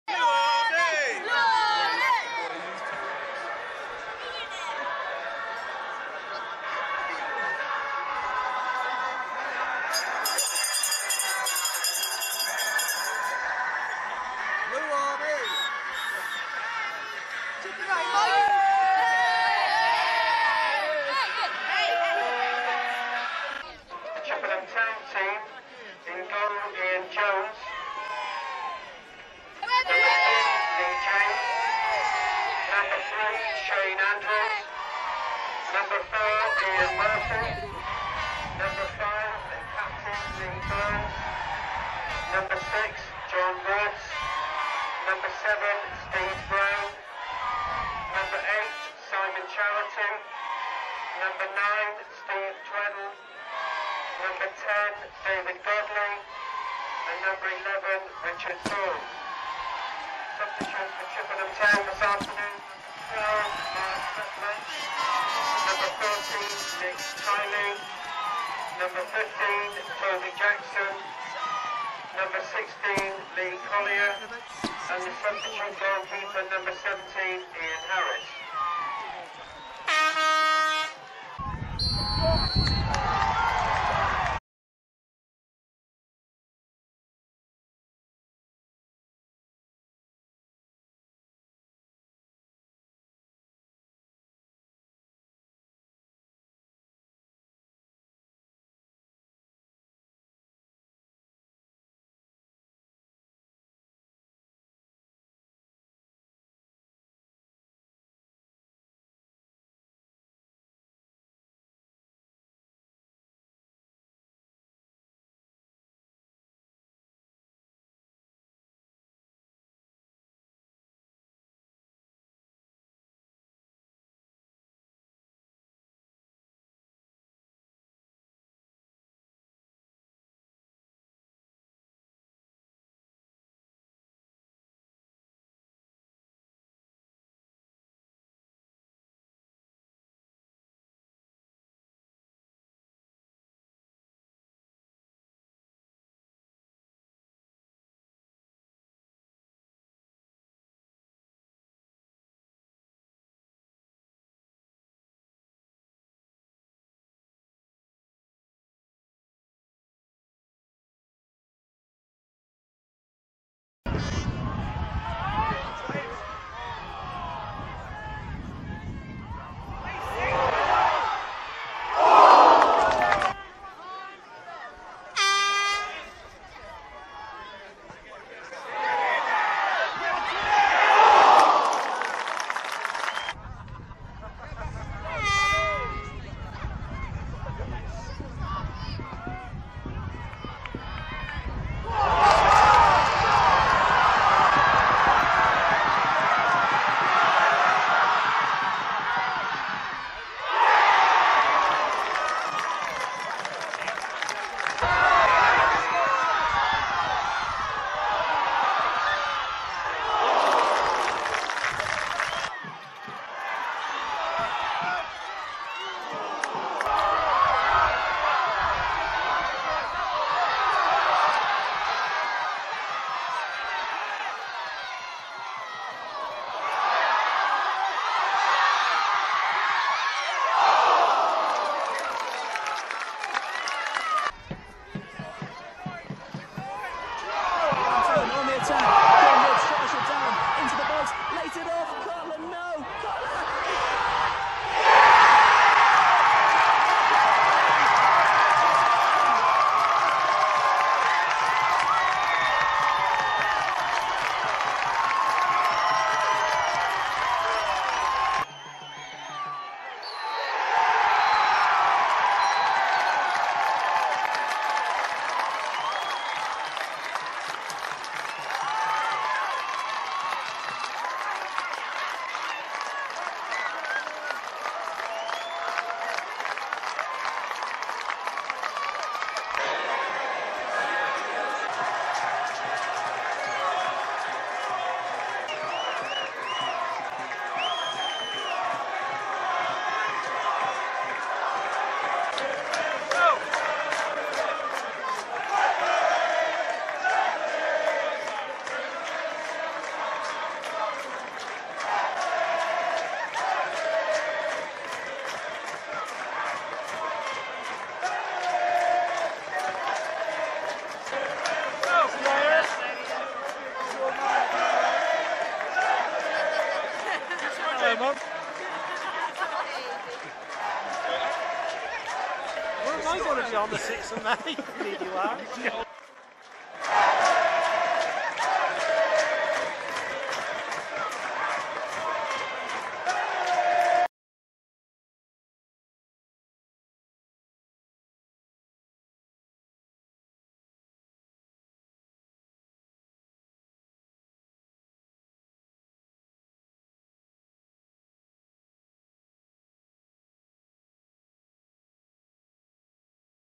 RETRO BLUEBIRDS | Chippenham Town vs Vauxhall Motors | 1999/00 FA Vase
We look back to 2000, as the Bluebirds look to make it to Wembley, but they first must get past Vauxhall Motors in the FA Vase.